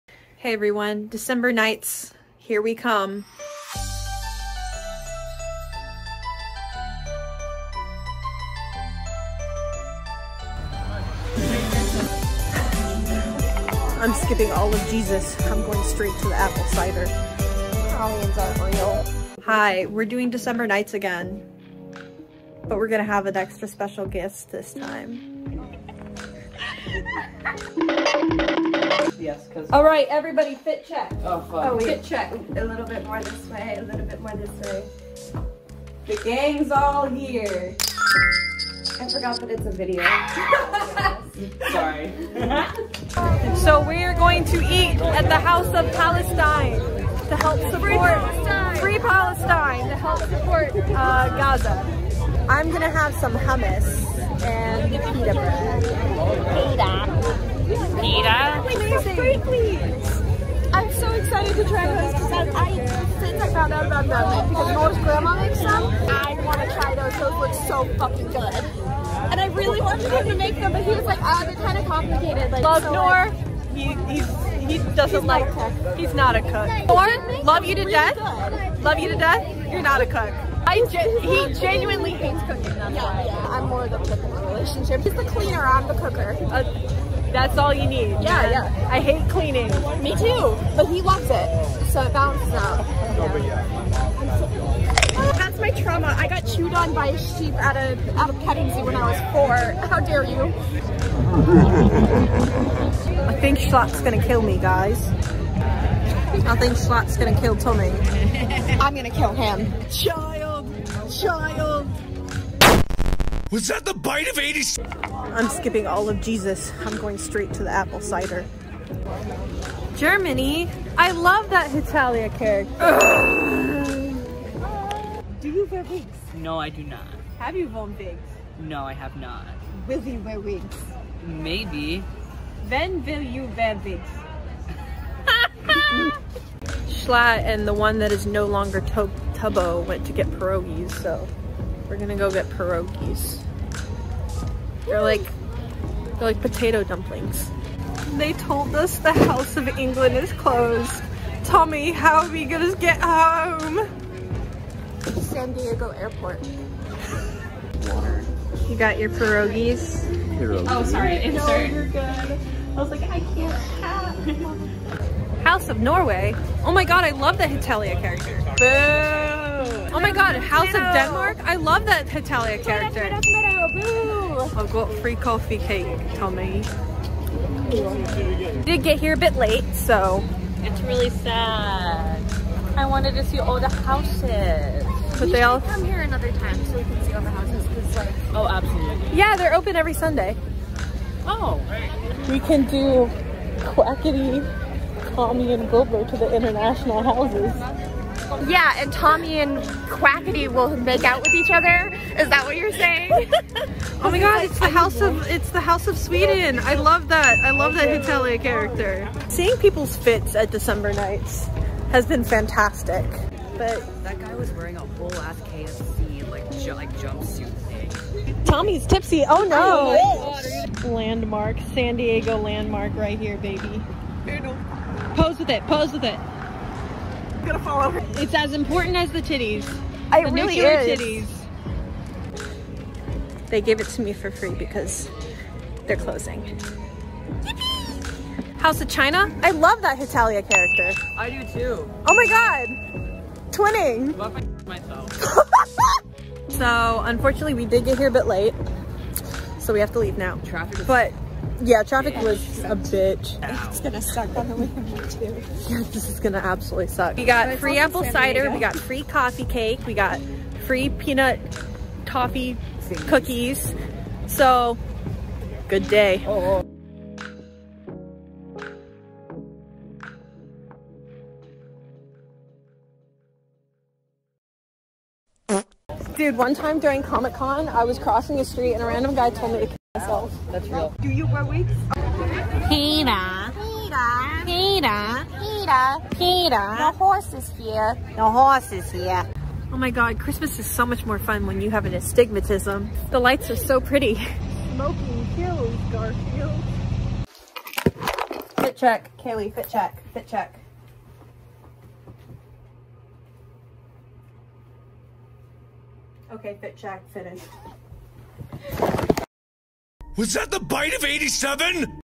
The Crime Boys and friends take on December Nights!
So me and my friends dressed as Tommy, Tubbo, Schlatt, and wilbur, and messed around in at December Nights in San Diego at Balboa Park.

Schlatt: Cherry

Wilbur: Me!!